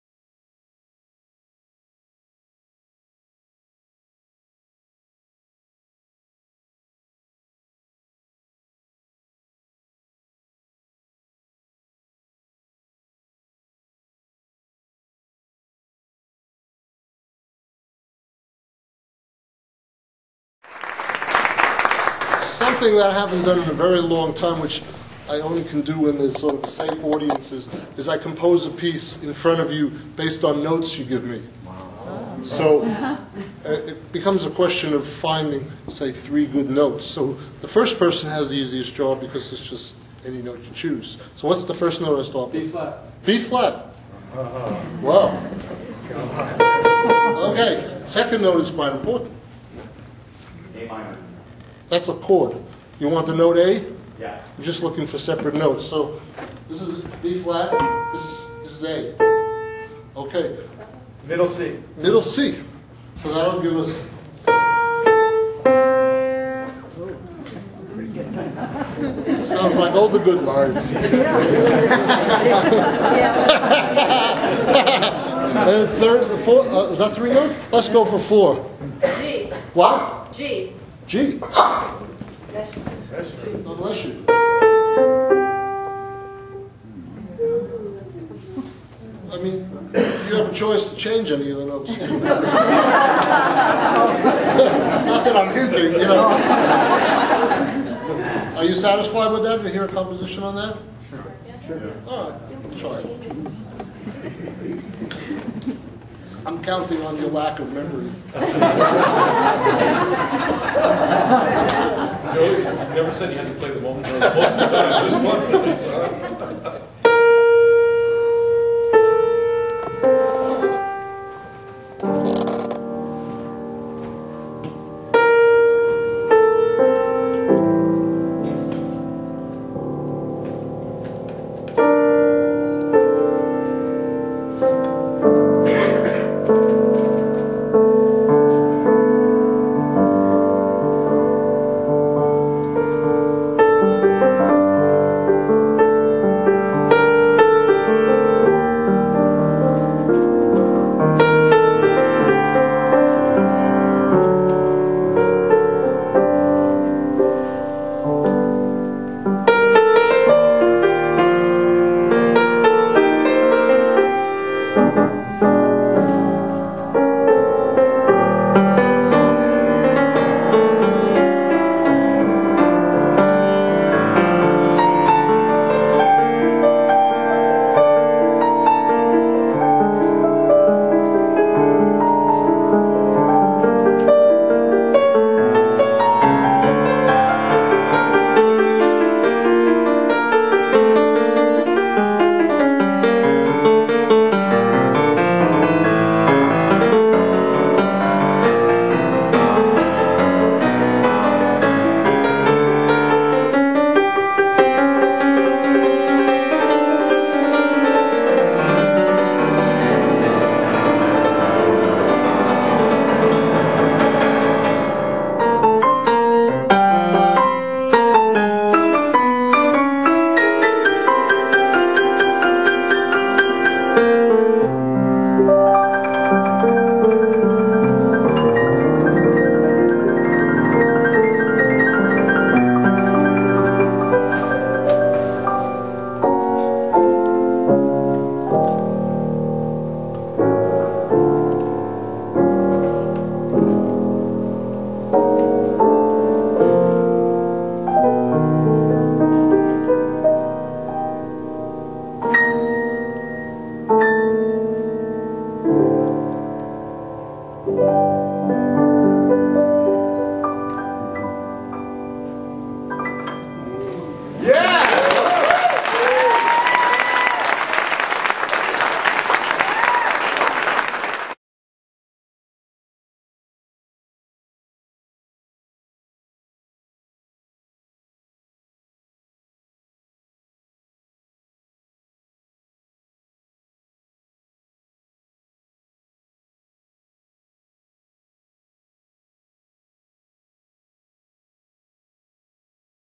Mike Garson - 4 Note Improvisation
Totally Garson, an evening with Mike Garson.  Mike Does a Q & A session. In this clip Mike has the audiance give him 4 notes from which he creates an original composition live.